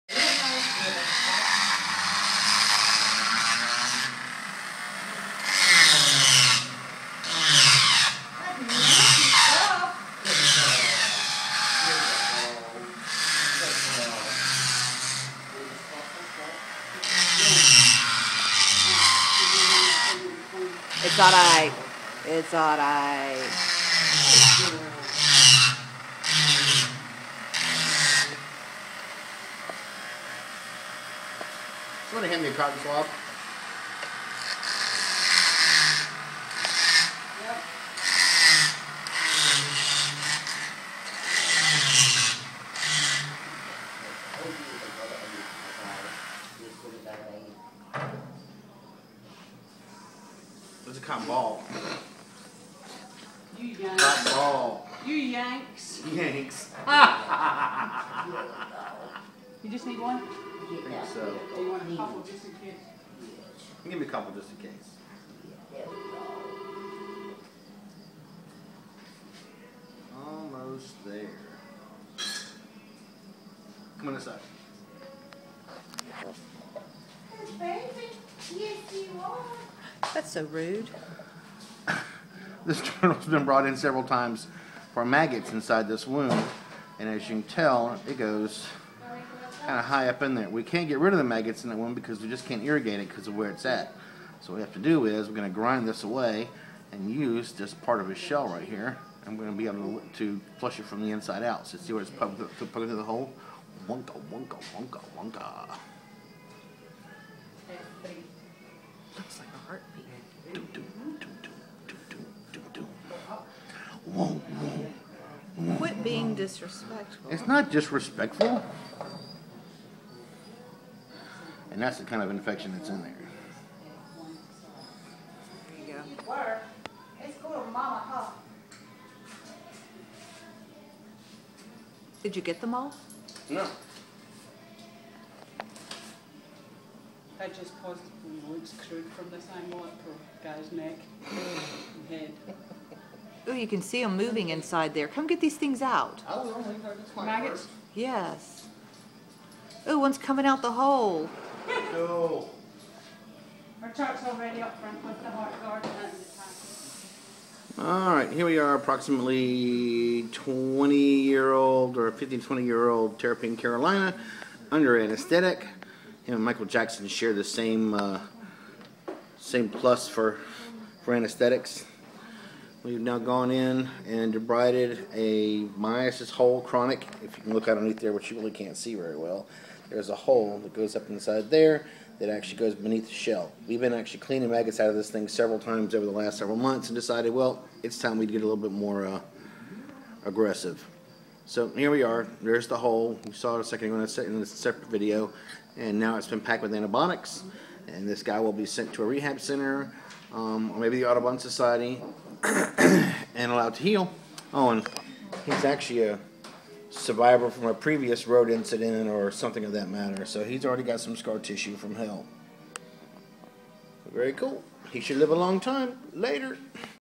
Maggots!!!! T carolina in for chronic myiasis and carapacotomy as treatment
Graphic! Beware!!! Maggots! T carolina in for chronic myiasis and carapacotomy as treatment